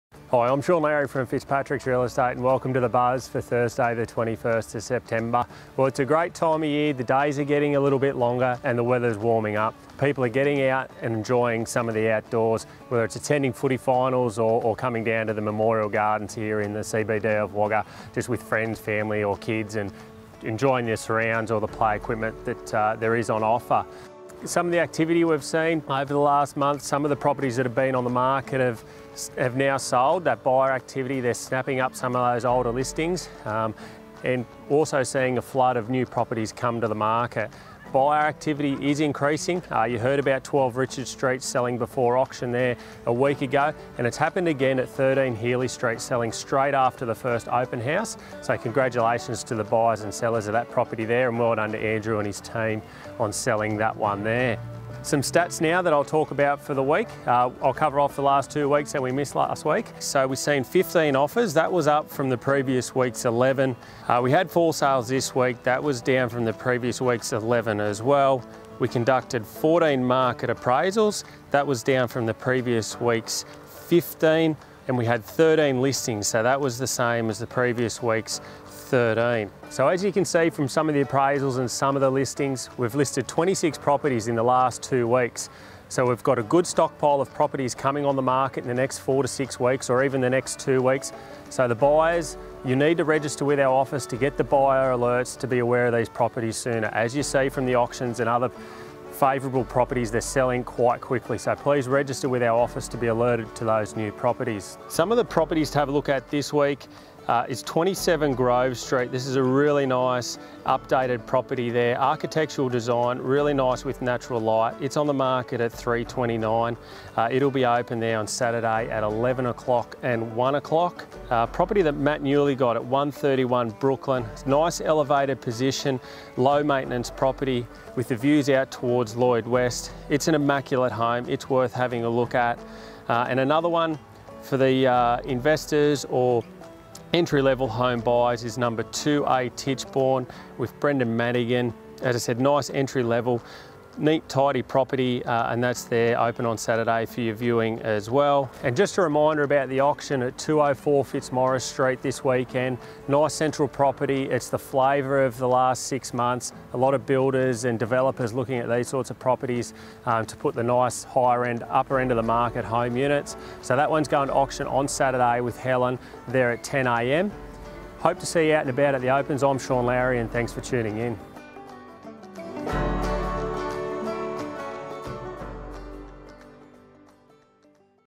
The Fitzpatricks Buzz Ep 62 - 21 September 2017
Fitzpatricks Director Shaun Lowry gives us a wrap of the latest listings and last week's stats from the Victory Memorial Gardens.

In this video:
27 Grove Street, Kooringal